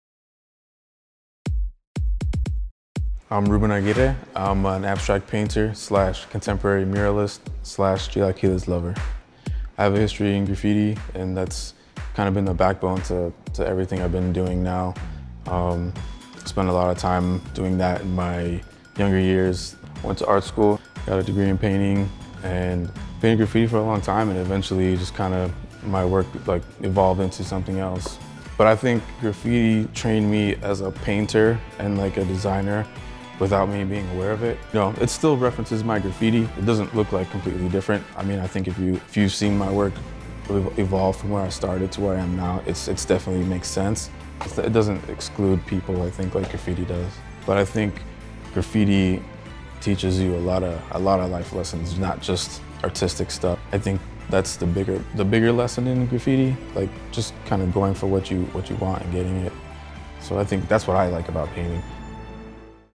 Artist to Watch: Ruben Aguirre | Sierra Mist Exquisite Collaboration - Chicago
Introducing Ruben Aguirre, abstract painter / contemporary muralist. He’s one of five talented artists who combined their styles to paint one Chicago billboard as part of our Exquisite Collaboration Project.